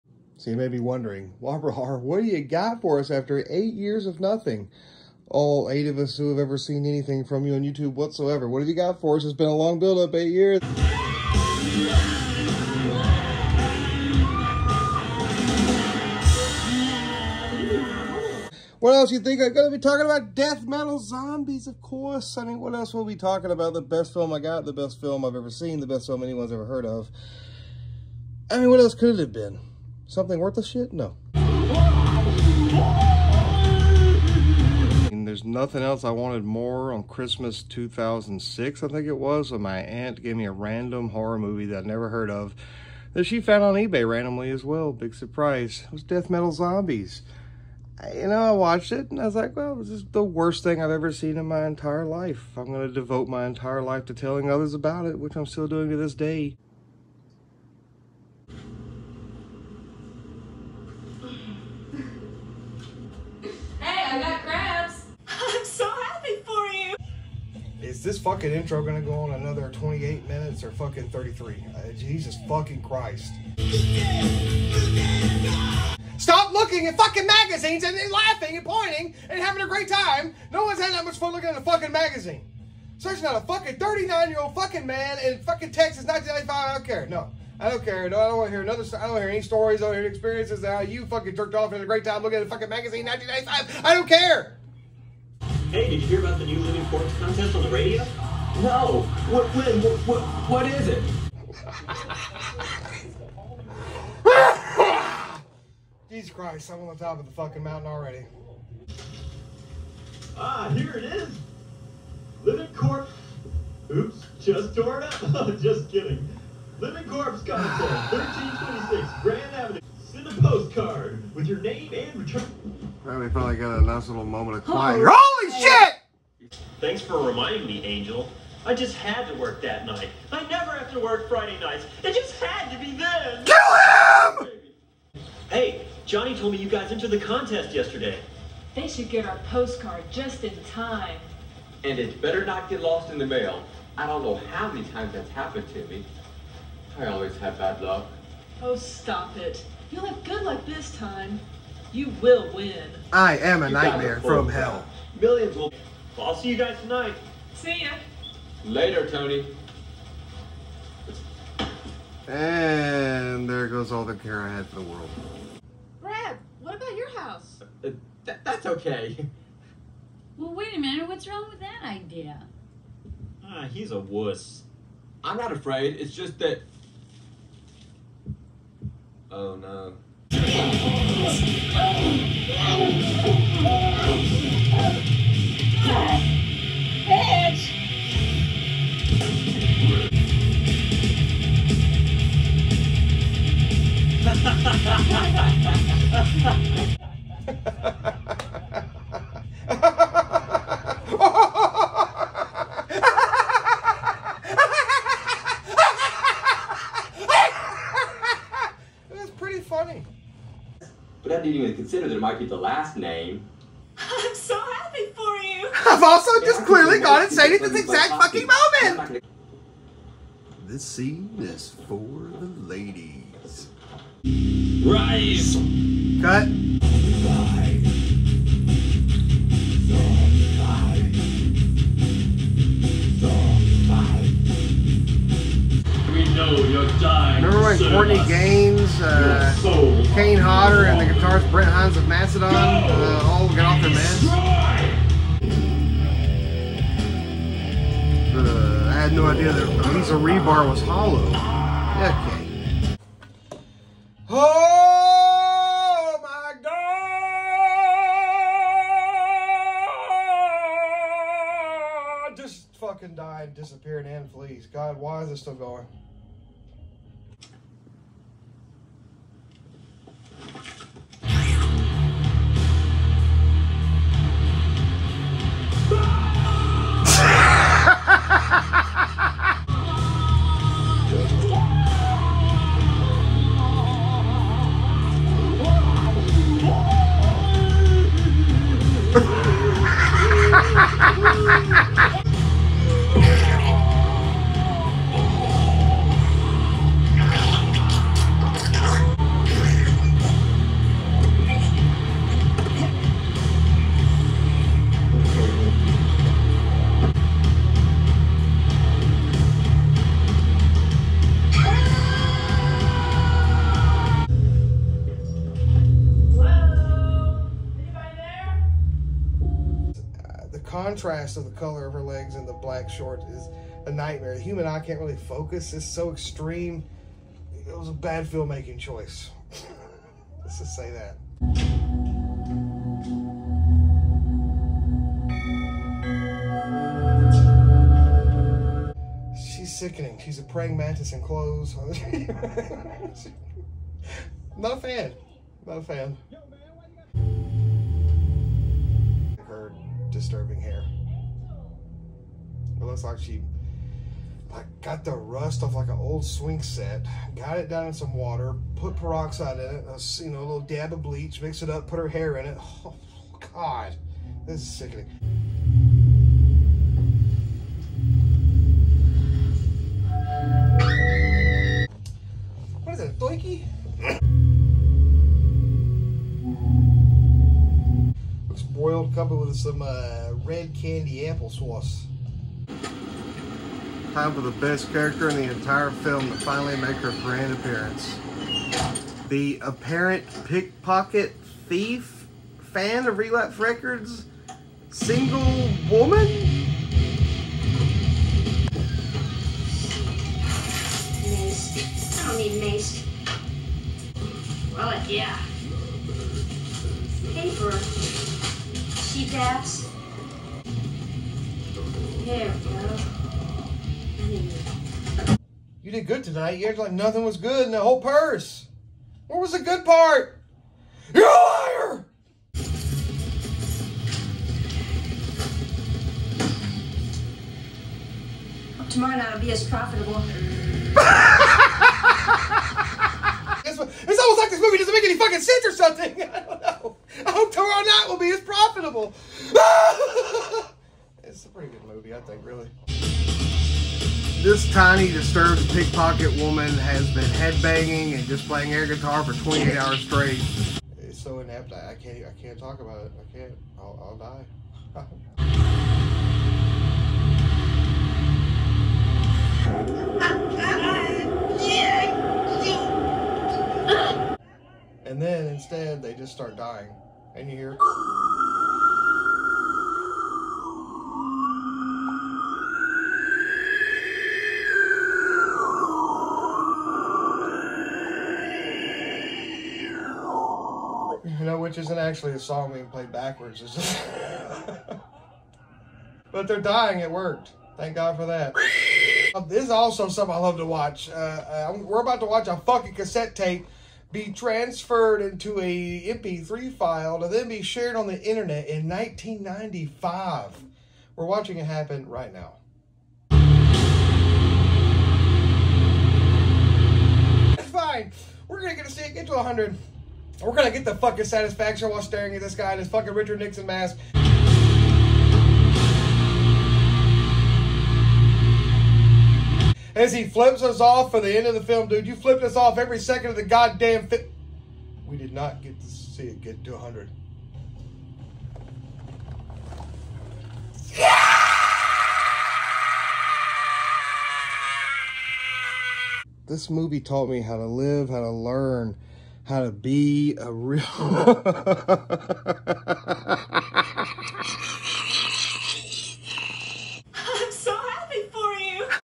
Back after 8 years just to talk about DEATH METAL ZOMBIES…
Death Metal Zombies. A landmark horror film in everyone’s heart. Join Brad, Tony and Angel in their fight against an evil death metal band who’s one of a kind cassette tape turns all who hear it into zombies. Budget: prob close to the same amount you can grab one of those 11 hour old rotisserie chickens at the Walmart checkout line for